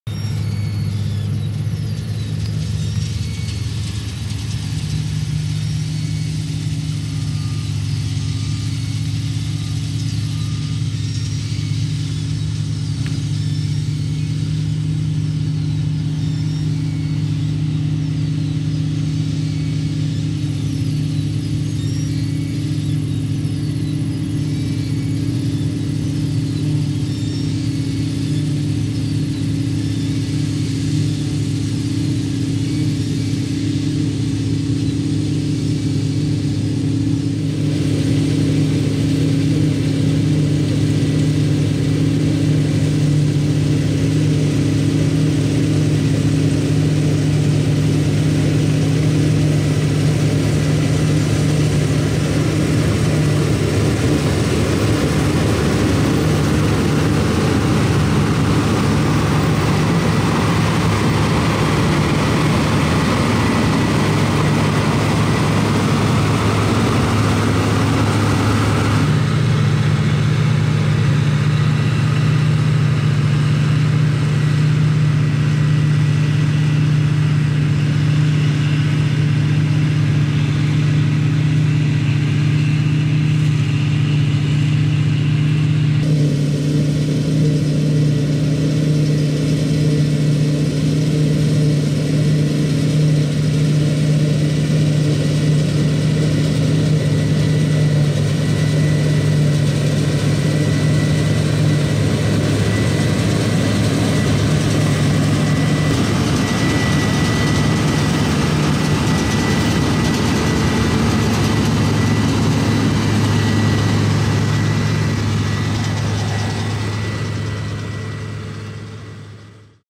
New Holland Mähdrescher | Combine Harvester | Free to use 4K footage - creative commons
Die Getreideernte hat begonnen. Rund um die Uhr arbeiten nun die Erntemaschinen auf den Feldern. Im warmen Licht der Abendsonne wird hier Gerste gemäht.
Mähdrescher CX8080 mit Geringhoff Schneidwerk HV660.
Die Produktion des CX 8080 wurde 2011 eingestellt und der Mähdrescher kostete zwischen 232.000 bis €295.000 Euro.

The grain harvest has started. The harvesting machines are now working in the fields around the clock. Barley is mowed here in the warm light of the evening sun.
Combine harvester CX8080 with Geringhoff cutting unit HV660.
Production of the CX 8080 was discontinued in 2011, the combine harvester costs between € 232,000 and € 295,000.

Edited with Adobe Premiere Pro and Adobe After Effects